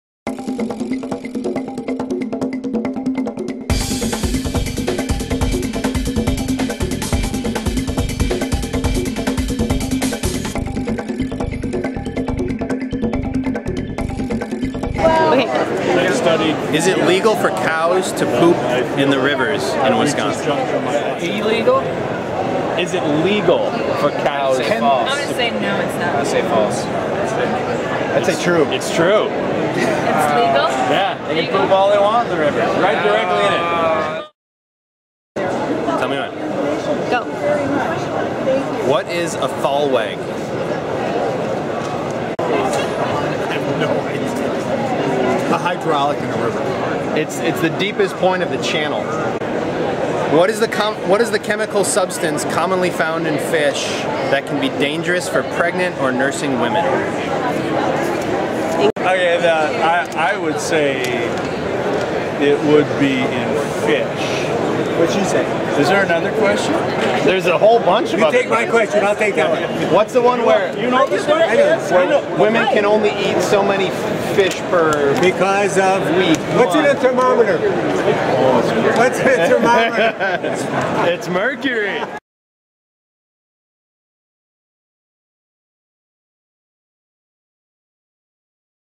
River Rat Pub Quiz: Canoecopia 2012
River Alliance of Wisconsin's booth at Canoecopia was transformed into a game show! Trivia questions about riverine geography, history and biology stumped paddlers at the World's Largest Paddlesports Exposition.